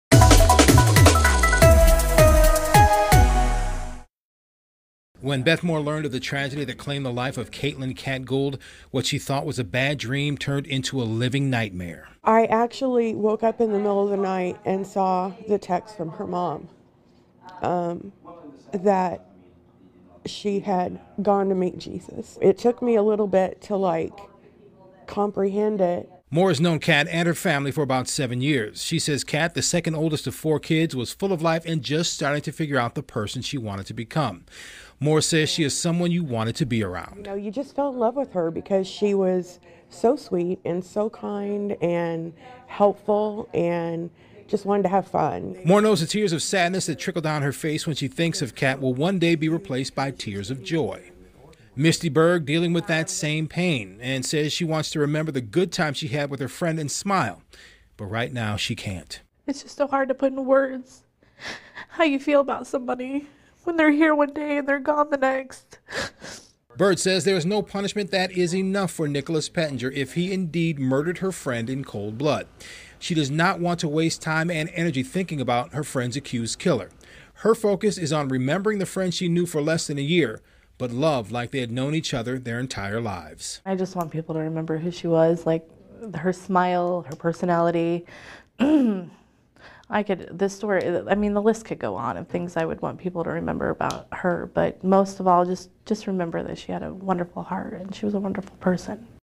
Friends remember victim in homicide investigation
Katelynn Rose Gould, 19, died after sheriff's deputies and paramedics found her in a home the 3200 block of E. Aurora Avenue in the Douglas Acres area on a domestic call.